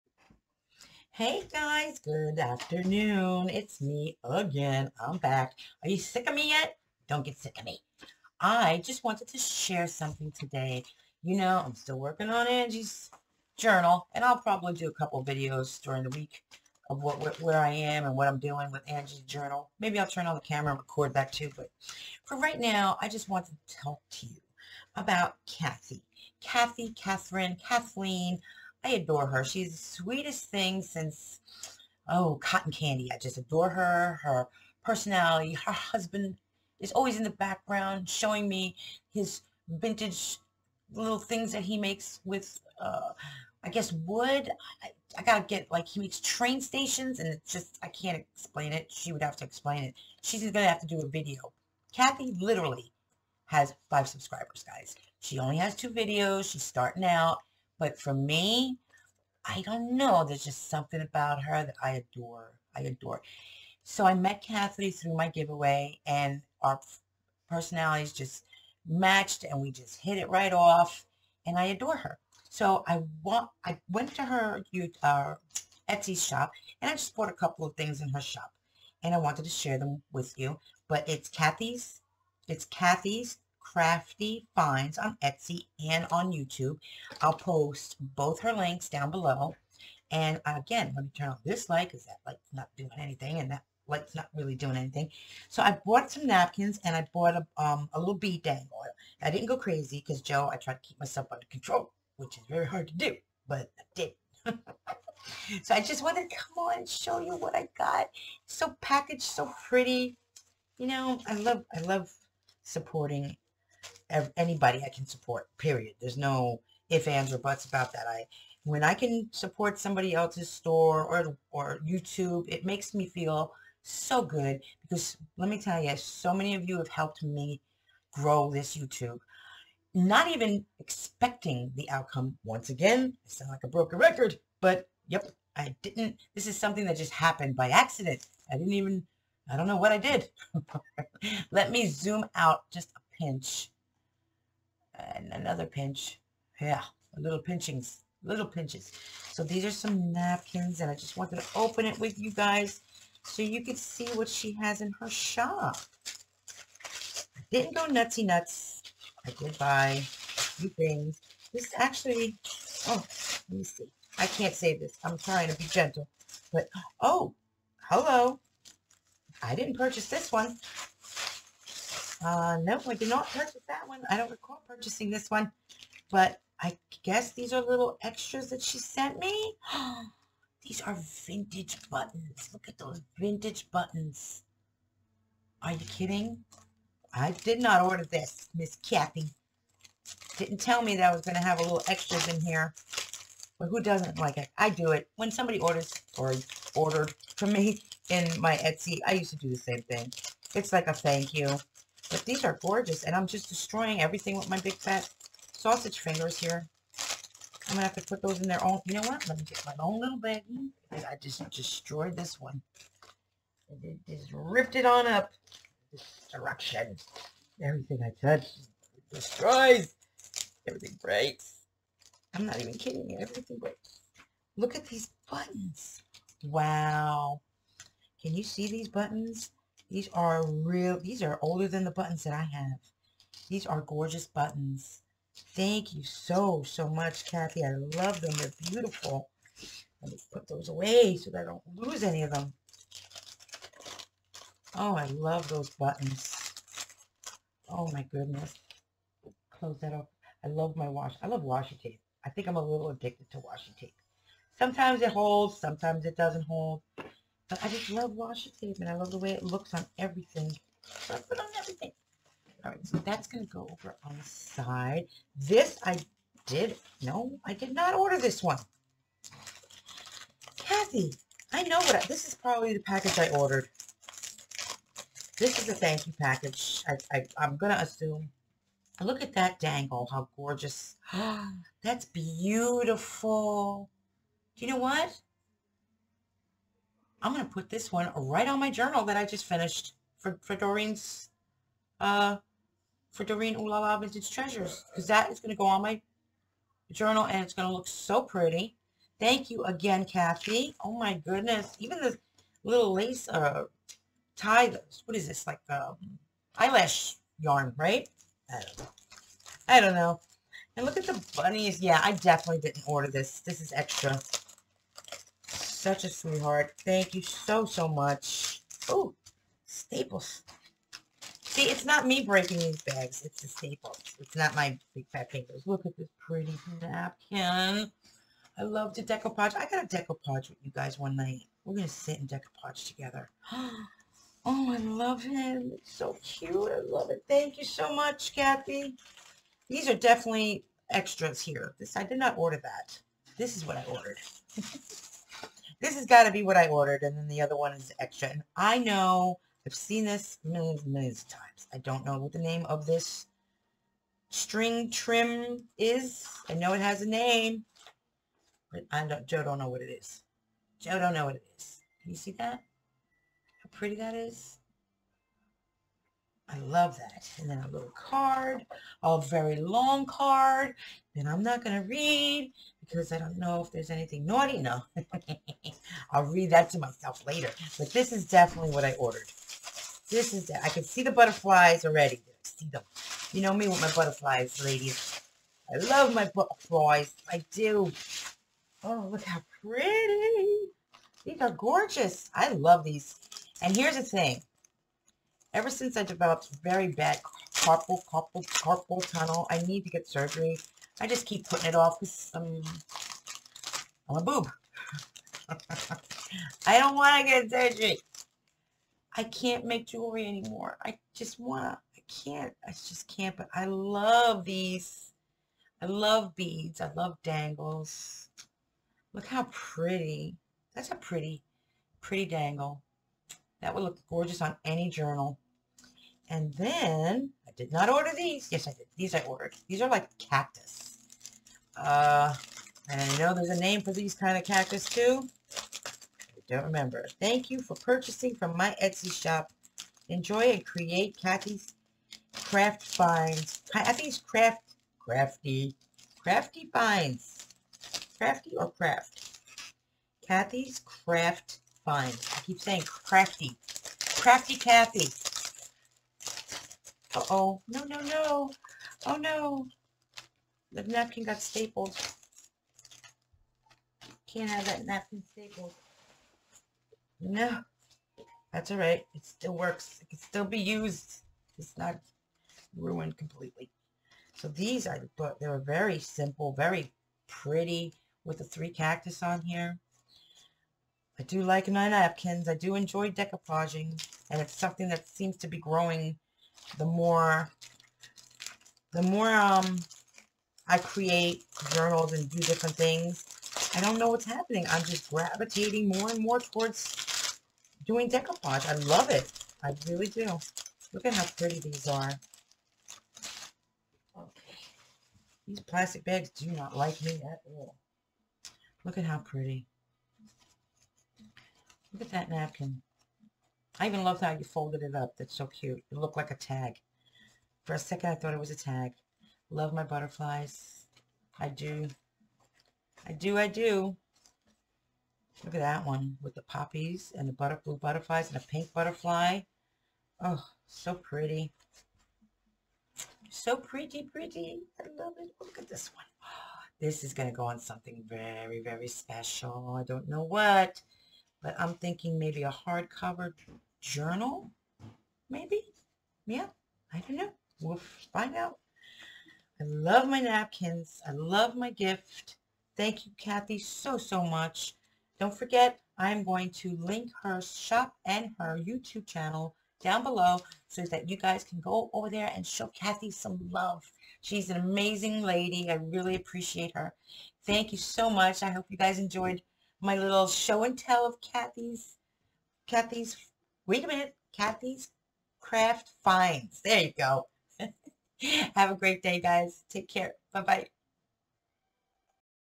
My Order From Cathys Craft Finds On Etsy
Hey Gals How is everyone's Wednesday going so far. I miss you all
So i just wanted to share what i purchased from Cathy's Etsy shop. Hope you enjoy. And if you would stop by her youtube and show her some love. I have some of the best people here with me and  your all so amazing at supporting each other. I adore you all so very much.
Thank you again Catherine :-) XOXOX

My Videos are intended for Adults 18 and older.